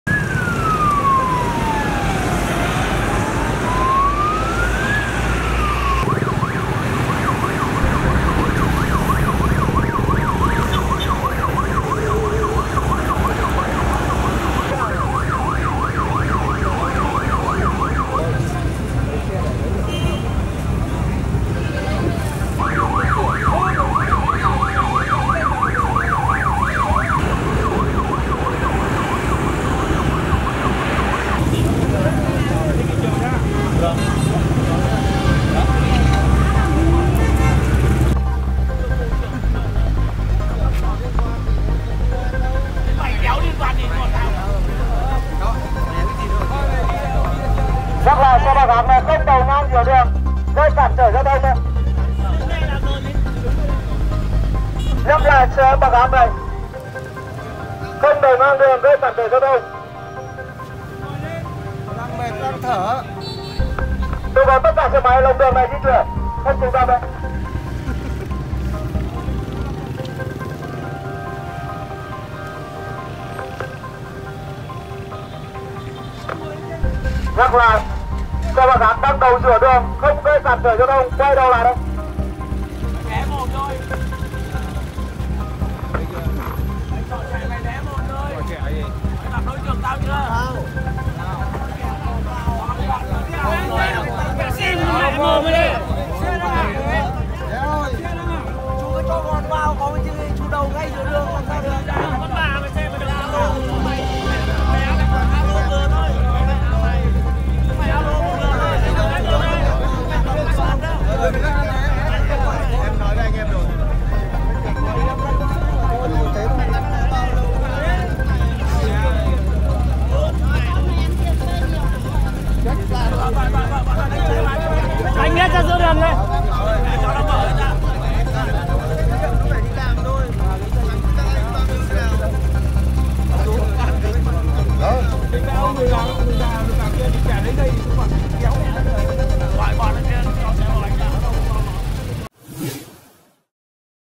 Tăng cường biện pháp ngăn chặn gây rối tại VFF nhằm mua vé chung kết AFF Cup 2018
Sáng 11/12, trước trụ sở Liên đoàn bóng đá Việt Nam tiếp tục có rất đông người dân tụ tập hi vọng có được tấm vé xem trận chung kết AFF Cup của tuyển Việt Nam.